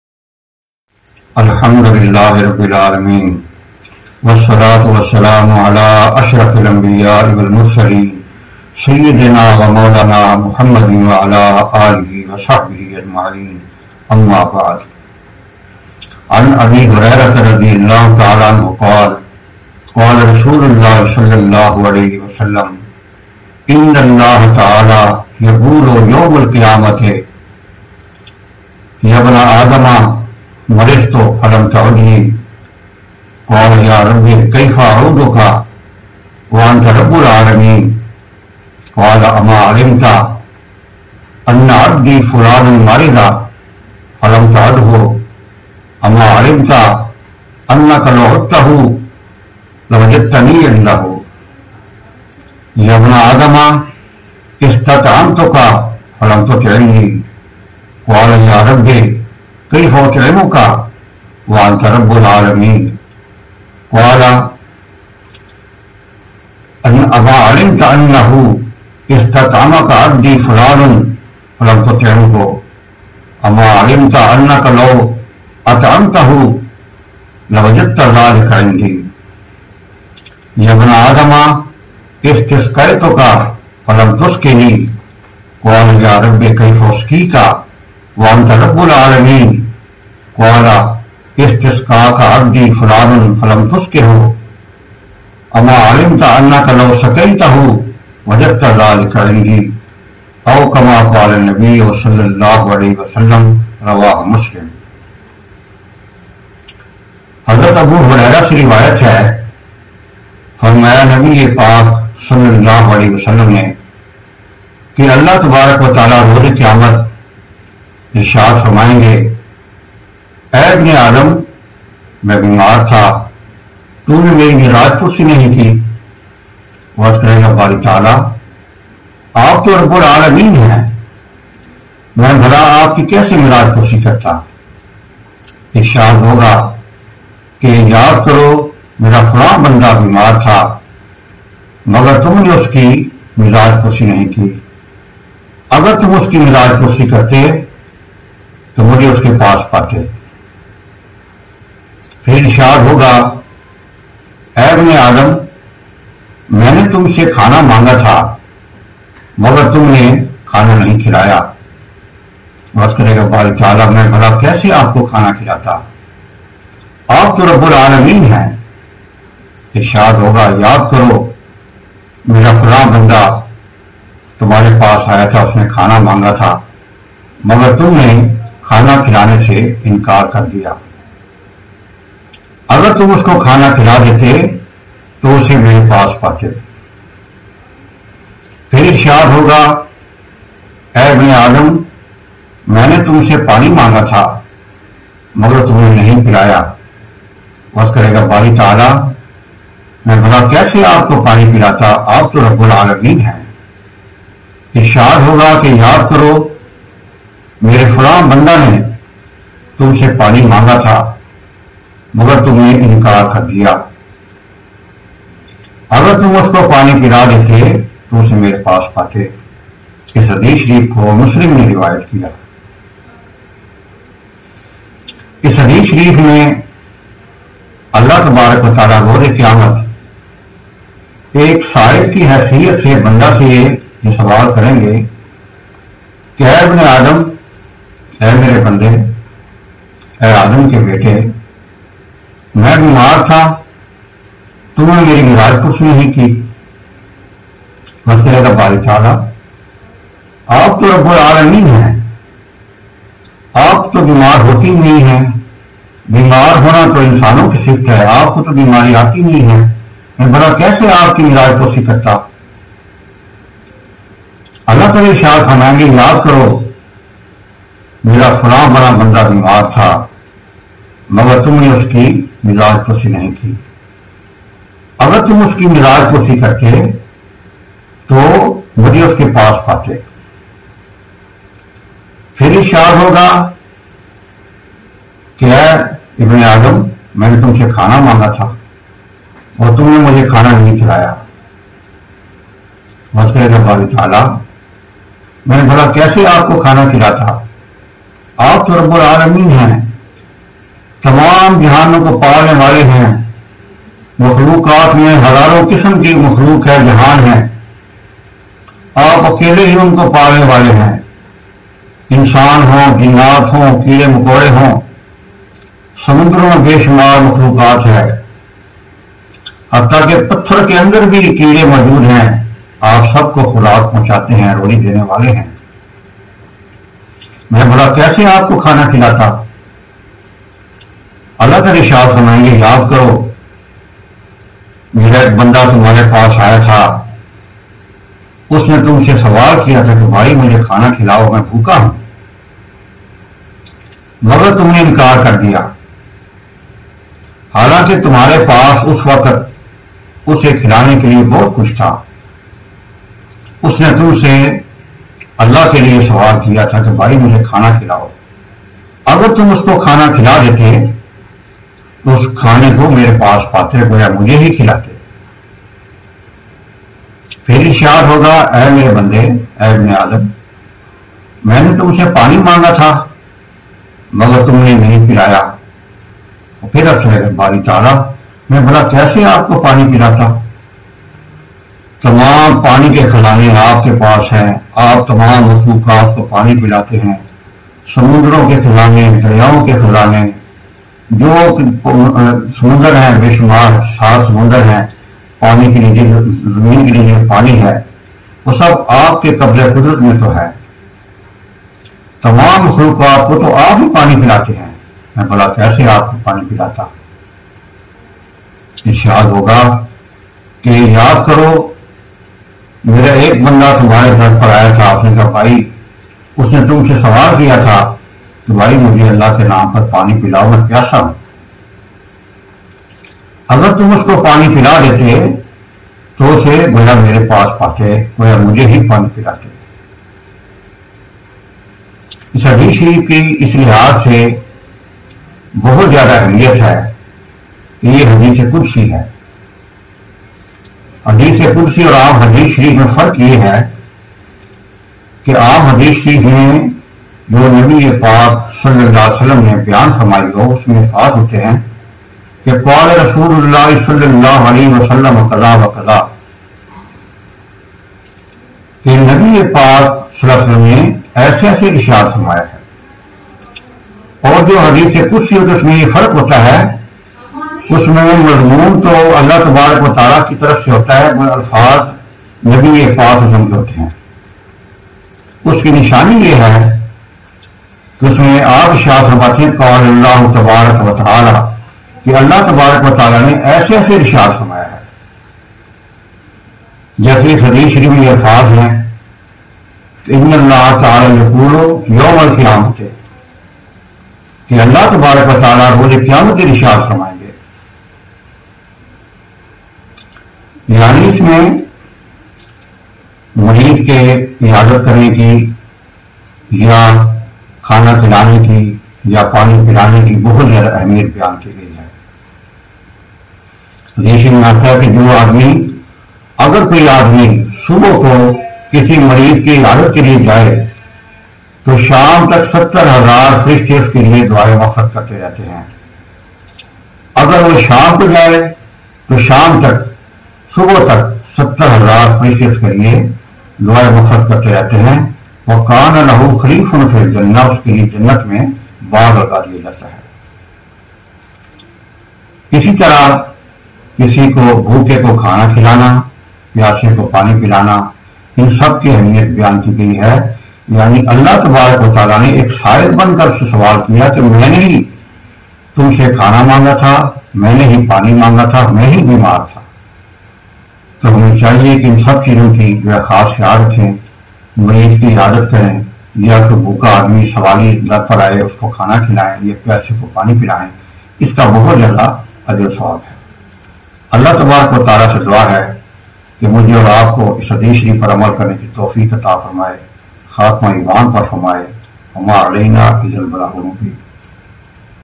'O' son of Adam, I was hungry but you did not feed me
Respected viewers, this Hadith reveals that Allah Almighty will ask at the Day of Judgement from a faithful believer that He  was ill the but the believer did not visit to Allah!